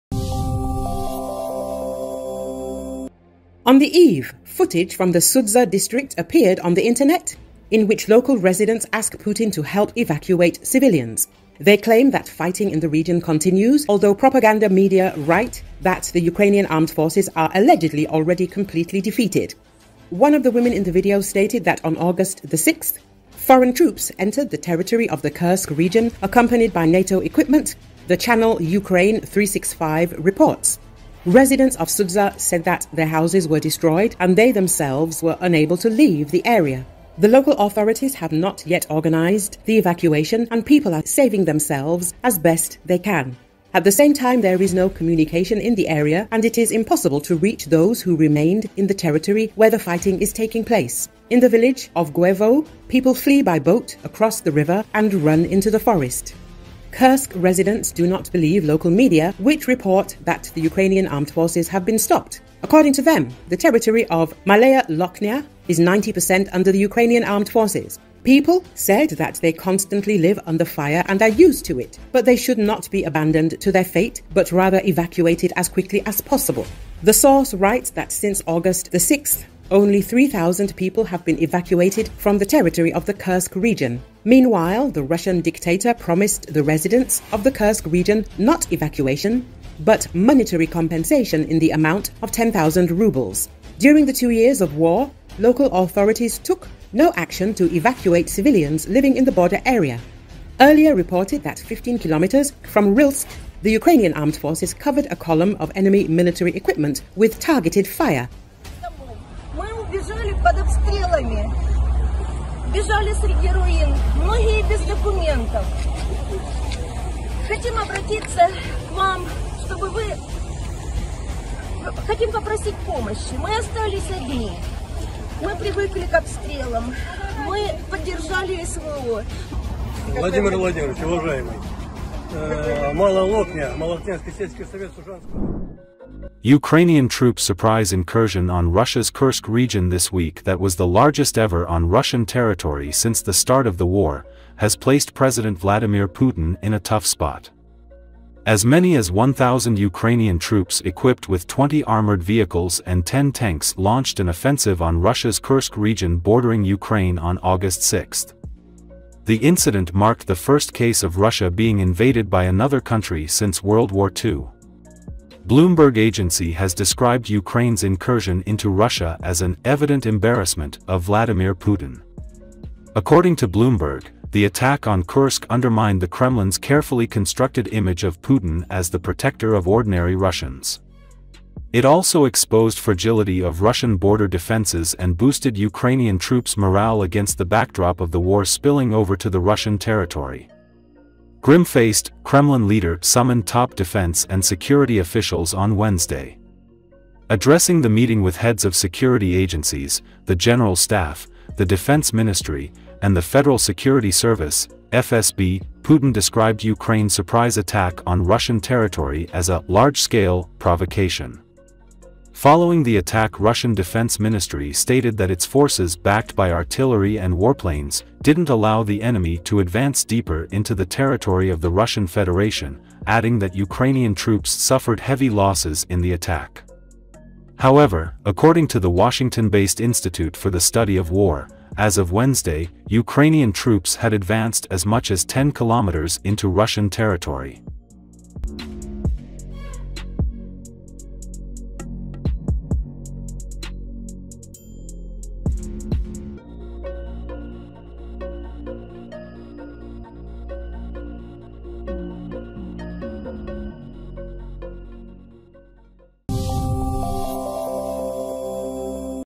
"Civilians are dying because of this lie" - Kursk region residents record video message to Putin
Footage from the Sudzha district appeared on the Internet, in which local residents ask Putin to help evacuate civilians. They claim that fighting in the region continues, although propaganda media write that the Ukrainian Armed Forces are allegedly already "completely defeated." One of the women in the video stated that on August 6, "foreign troops" entered the territory of the Kursk region accompanied by "NATO equipment," the channel " Ukraine 365 " reports. Residents of Sudzha said that their houses were destroyed and they themselves were unable to leave the area.

The local authorities have not yet organized the evacuation, and people are saving themselves as best they can. At the same time, there is no communication in the area and it is impossible to reach those who remained in the territory where the fighting is taking place. In the village of Guevo, people flee by boat across the river and run into the forest. Kursk residents do not believe local media, which report that the Ukrainian Armed Forces have been stopped. According to them, the territory of Malaya Loknya is 90% under the Ukrainian Armed Forces. People said that they constantly live under fire and are used to it, but they should not be abandoned to their fate, but rather evacuated as quickly as possible. The source writes that since August 6, only 3 thousand people have been evacuated from the territory of the Kursk region. Meanwhile, the Russian dictator promised the residents of Kursk region not evacuation, but monetary compensation in the amount of 10 thousand rubles. During the two years of war, local authorities took no action to evacuate civilians living in the border area. Earlier reported that 15 km from Rylsk the Ukrainian Armed Forces covered  a column of enemy military equipment with targeted fire.